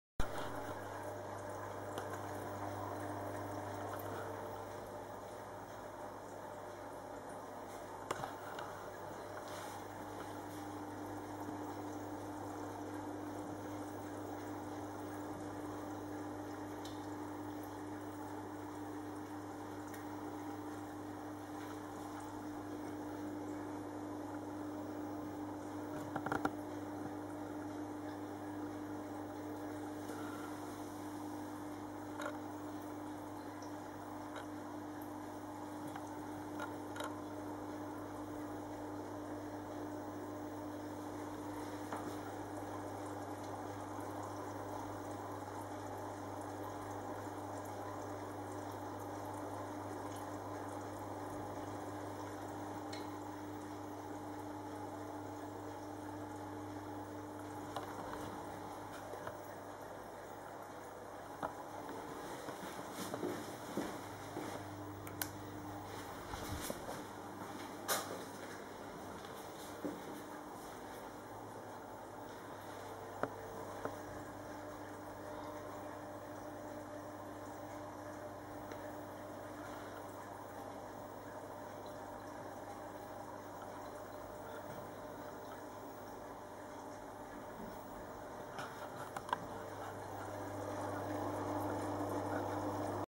new 22g fish tank
just finish instaling my new fish tank ; filter,lights,decorations. tomorow , my fishes will enjoy there new living world ;)